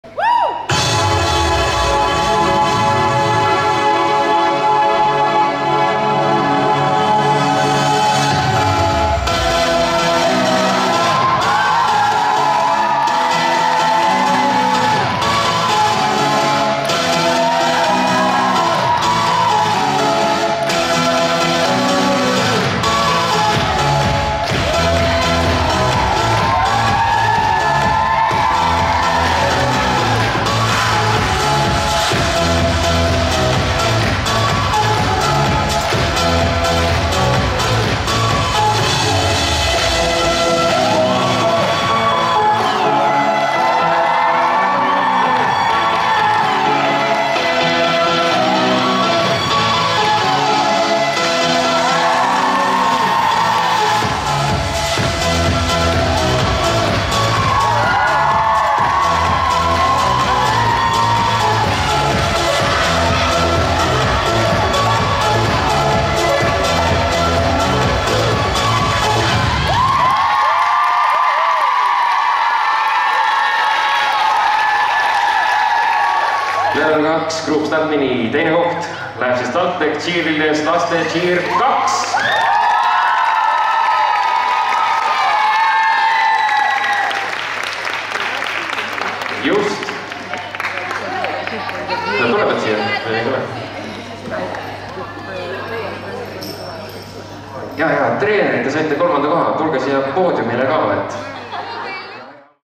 TalTech - Laste Cheer 2 🥈✨💖✨ Level 2 Group Stunt Mini | III EMV Cheerleadingus 2021
Estonian Cheerleading Championships 2021 | Cheerleading | Level 2 Group Stunt Mini | TalTech - Laste Cheer 2 2nd place - Harju County , Estonia - December 4, 2021.

TalTech särava Laste Cheer 2 suurepärane esitus (2. koht) 3. EMV Cheerleadingus Level 2 Group Stunt Mini kategoorias Kindluse Spordihoones - Rae vald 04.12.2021.

Suurepärase ürituse korraldustiim Eesti Cheerleadingu Liit - Estonian Cheerleading Union, TalTech Cheerleaders, Selver Tallinn DanceTeam ja SaChe Tantsutüdrukud 🥰🥰🥰

Mõnusat nautimist! :)